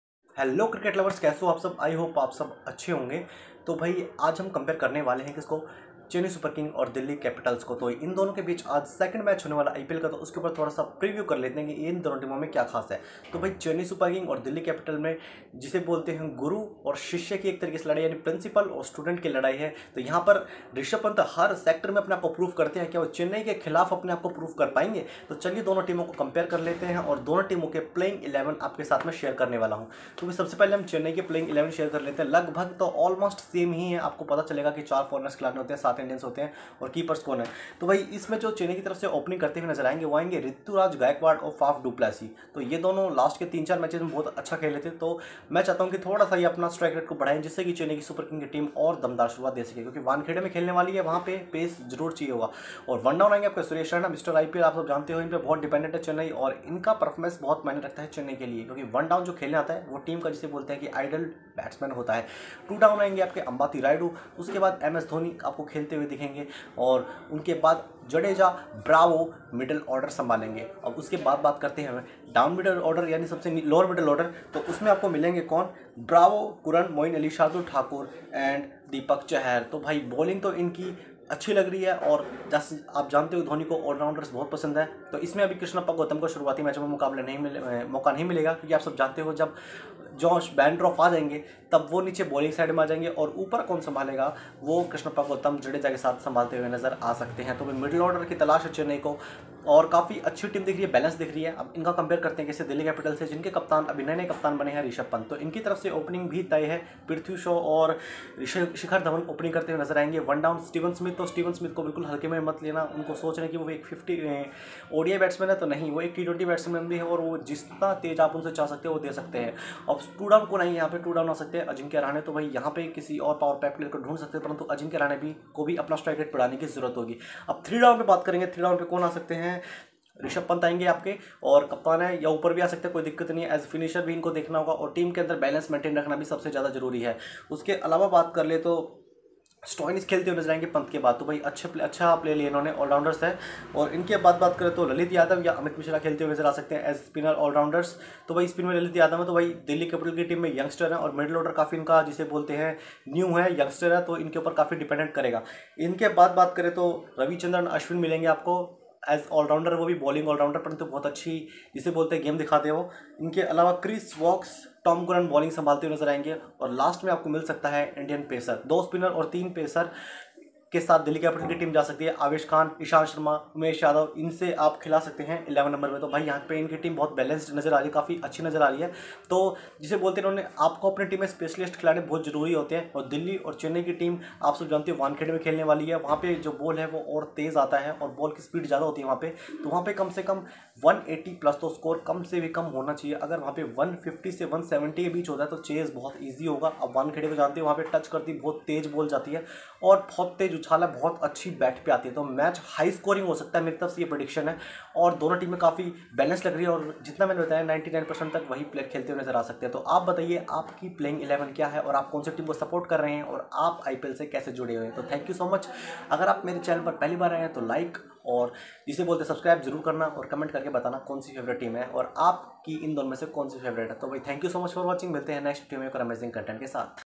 CSK V DC PREVIEW PLAYING 11 AND COMPARISON IPL 2021 | harvinder sports
About video
Discussion about csk v dc playing 11 and preview